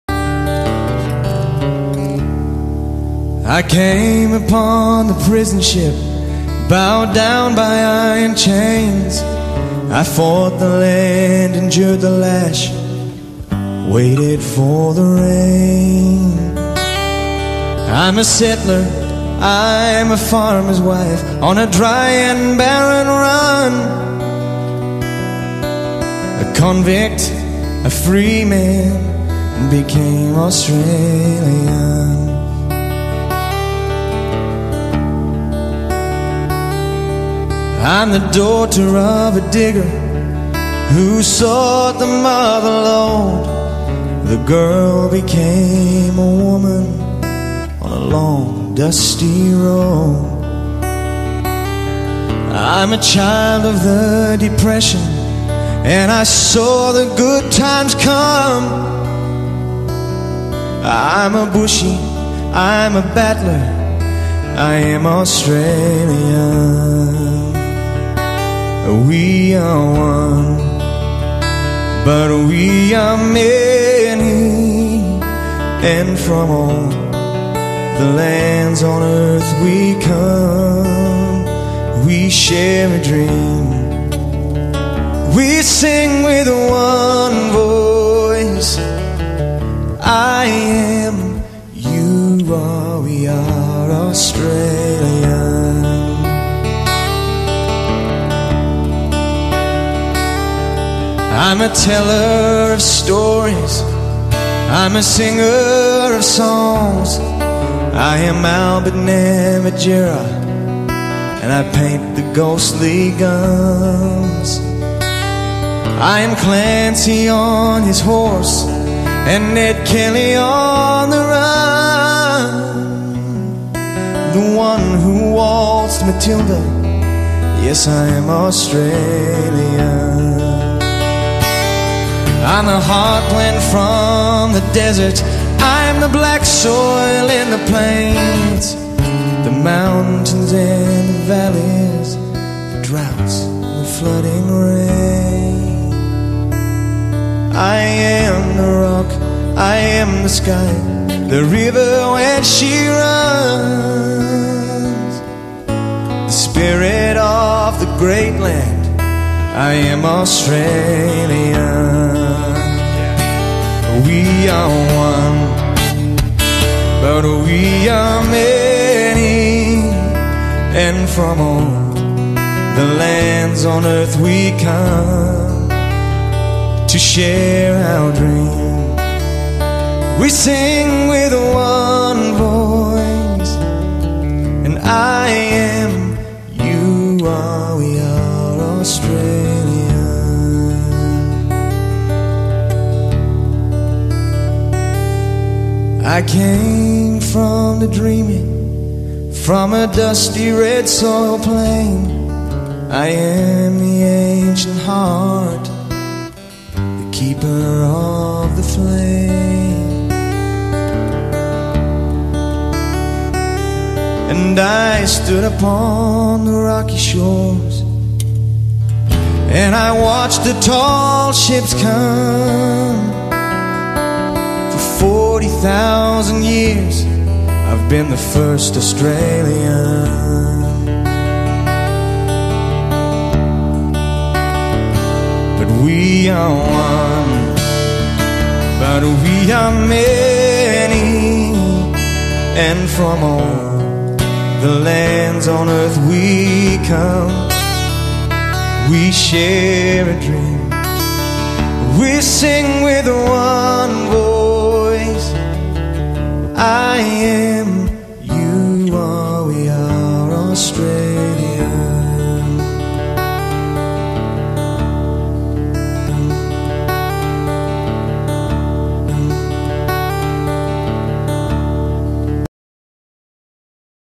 Ralph Honner Dinner Display
Video played at the 2011 Ralph Honner Oration Dinner paying tribute to the Kokoda Diggers and Fuzzy Wuzzy Angels. A special request from Rev. Peter Holloway who delivered the leadership oration on the night.